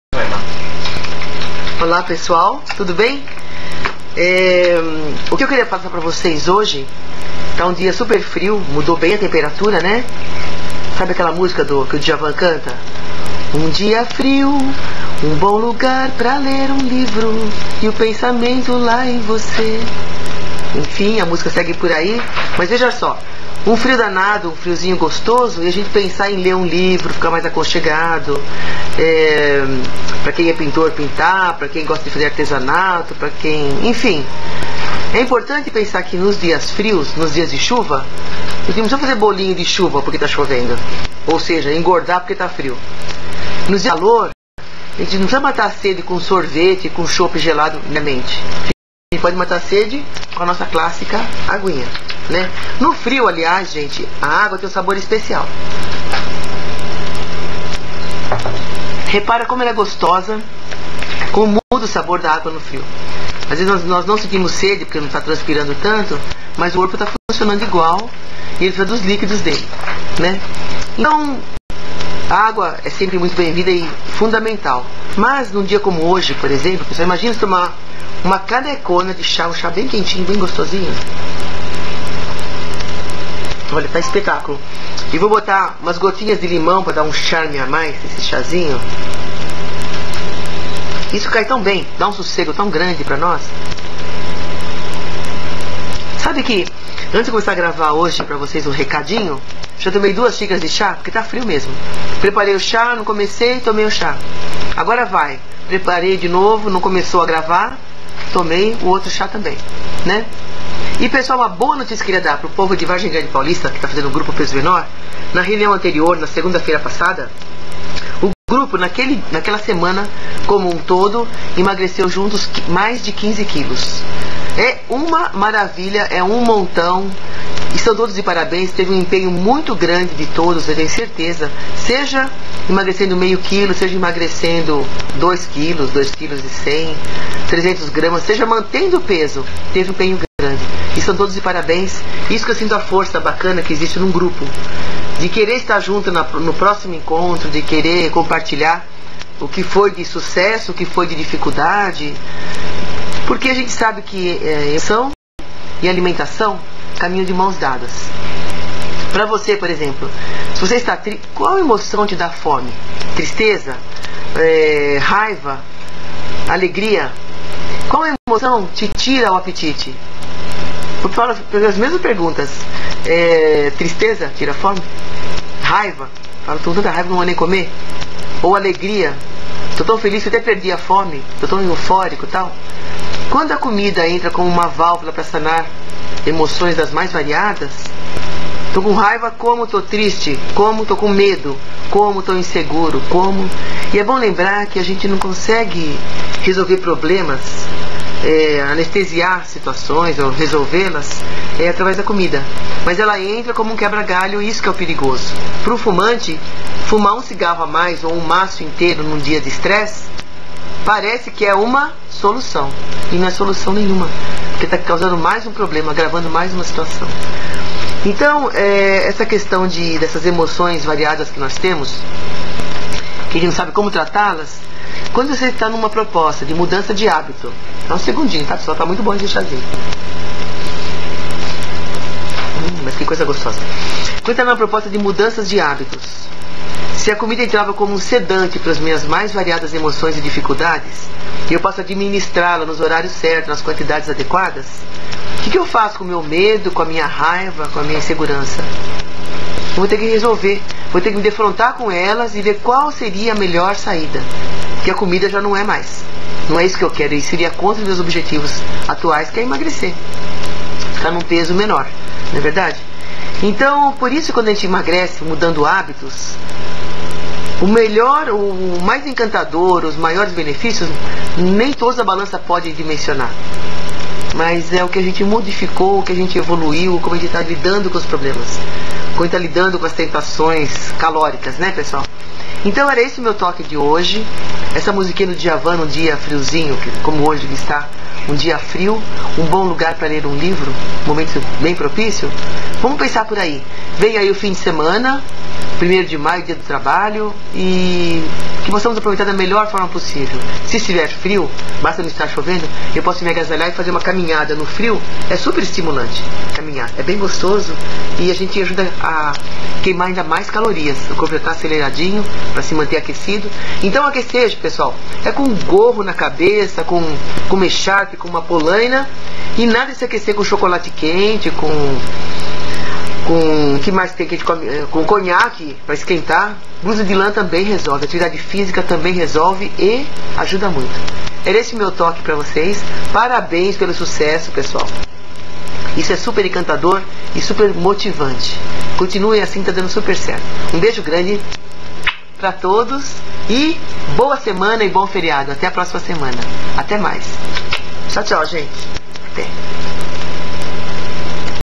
Recado da Marisa #3 | Semana de 23.04 a 27.04
Recado da semana de 23 para os participantes dos grupos.

Pessoal, decidi fazer um vídeo mais longo e deixá-los semanais.
Espero que gostem e curtam este friozinho que está chegando.

Grande beijo, Marisa.

Obs: Hoje o vídeo está um pouco melhor, para semana que vem será com a qualidade dos anteriores.

Reorientação Alimentar & Mudanças de Hábitos

Vargem Grande Paulista: Reuniões Segundas-feiras, às 19h00, no condomínio Haras Bela Vista.
Cotia: Reuniões Quartas-feiras, às 19h00, na escola de inglês CNA (próximo ao Fórum)